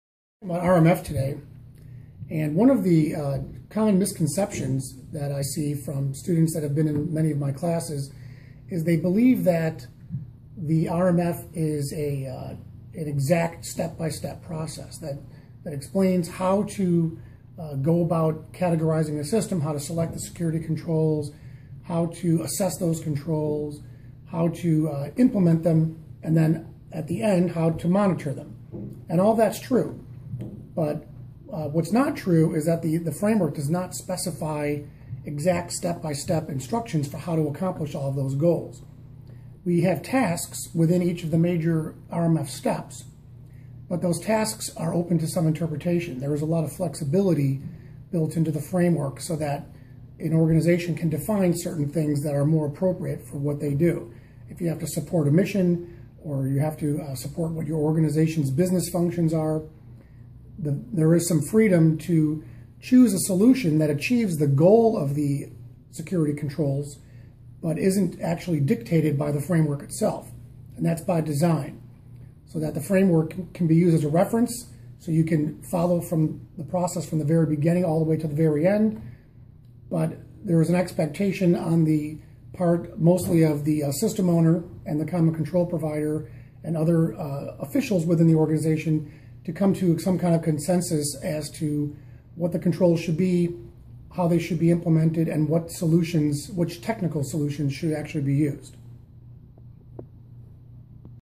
Is RMF an Exact Step-by Step Guide?
Dean Pompilio, RMF Instructor, breaks down a common misconception about Risk Management Framework and its intention to guide rather than dictate decision making.